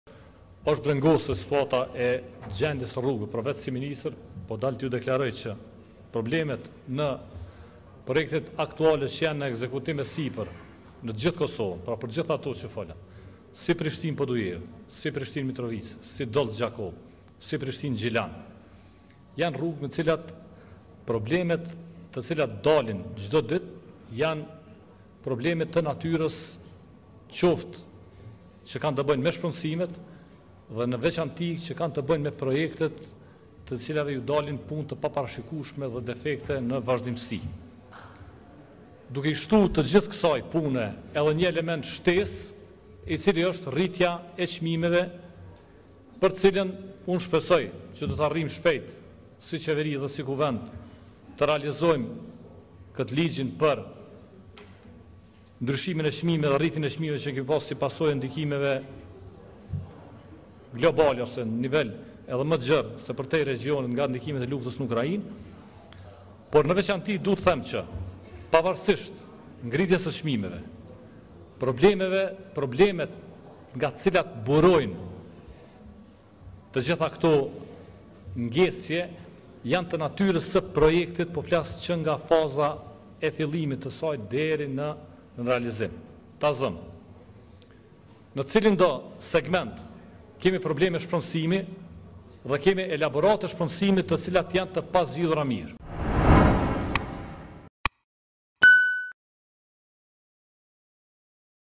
Ministri Aliu arsyetohet për vonesën në rrugën Prishtinë-Podujevë
Ministri i Mjedisit, Planifikimit Hapësinor dhe Infrastrukturës, Liburn Aliu është arsyetuar për vonesat në asfaltimin e rrugëve.